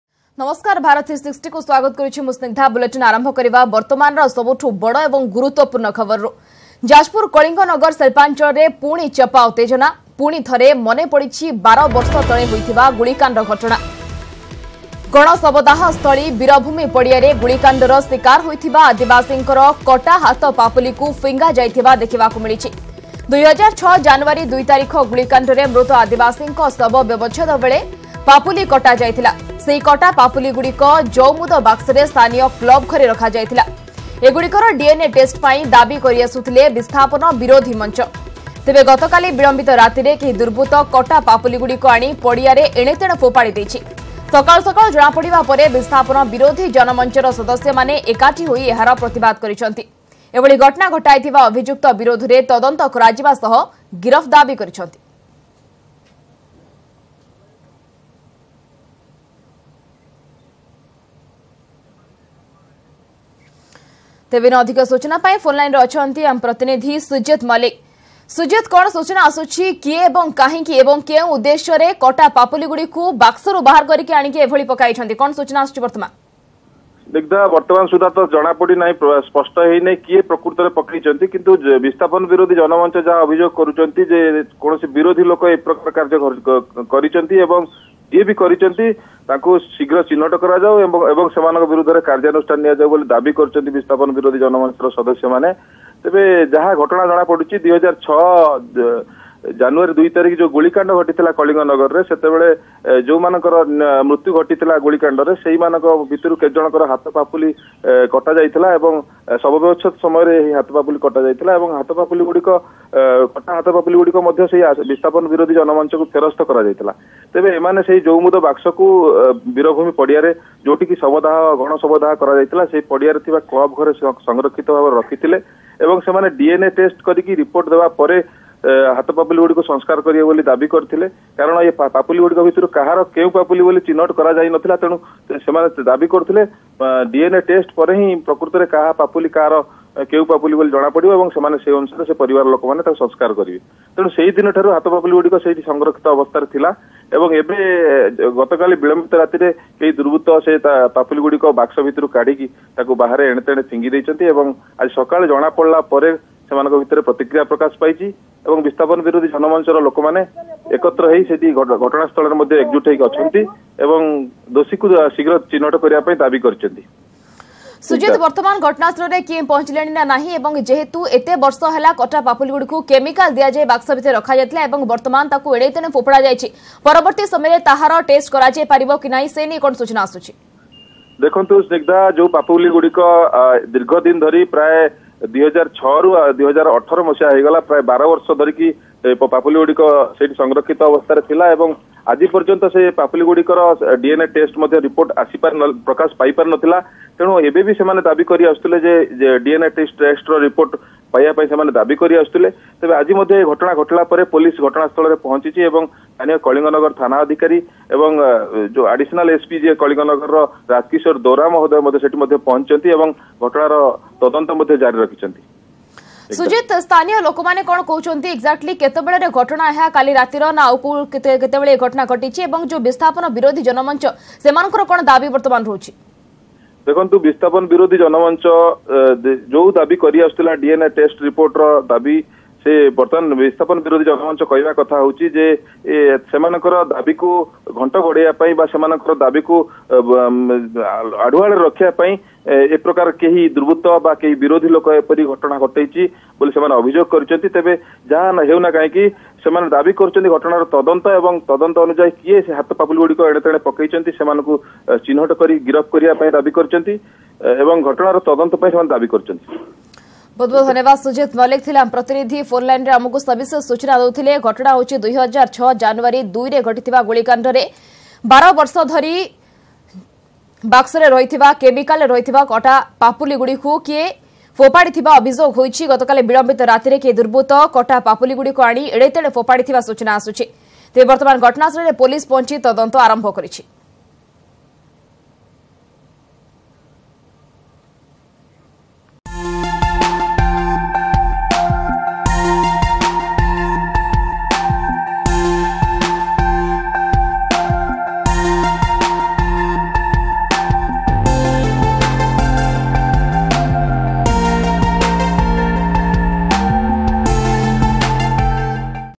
Jajpur: କଳିଙ୍ଗନଗର ଶିଳ୍ପାଂଚଳରେ ଉତ୍ତେଜନା,ପଡିଆରେ ପଡିଲା କଟା ହାତ ପାପୁଲି | BHARAT 360
କଳିଙ୍ଗନଗର ଶିଳ୍ପାଂଚଳରେ ଉତ୍ତେଜନା,ପଡିଆରେ ପଡିଲା କଟା ହାତ ପାପୁଲି.୨୦୦୬ ଗୁଳିକାଣ୍ଡରେ ମୃତ ଆଦିବାସୀଙ୍କ ପାପୁଲି କଟା ହୋଇ ବାକ୍ସରେ ରଖାଯାଇଥିଲା.